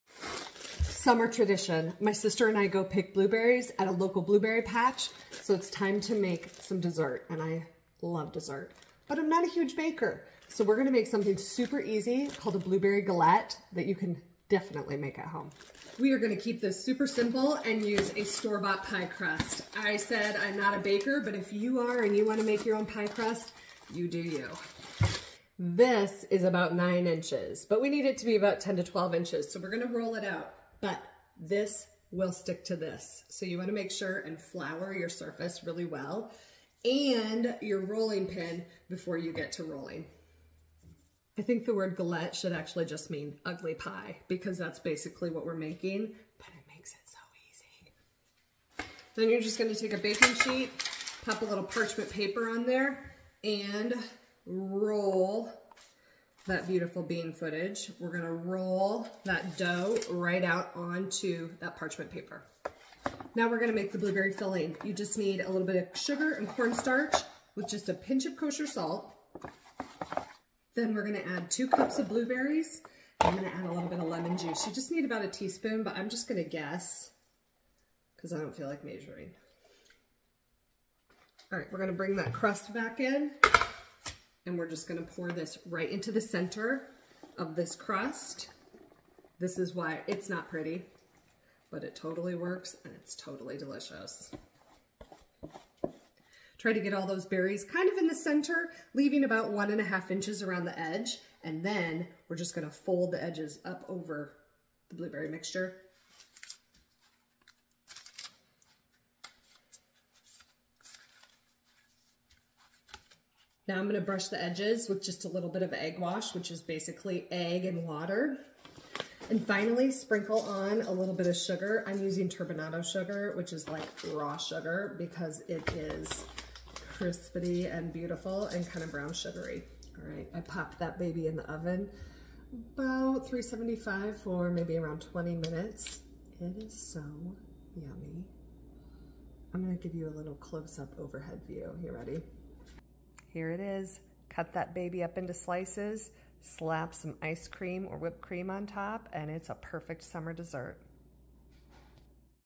Blueberry Galette Recipe
If you are not a pro baker, then make a delicious Blueberry Galette instead of a pie! It's so much easier! Perfect for all the summer berries.

TIMESTAMPS:
0:00 Intro - I picked a lot of blueberries!
0:28 rolling out the dough
0:48 prepping your baking sheet
1:07 making the blueberry filling
1:27 putting the galette together and folding the edges.
1:56 egg wash and sugar sprinkle
2:12 baking and final dish
2:23 close up of galette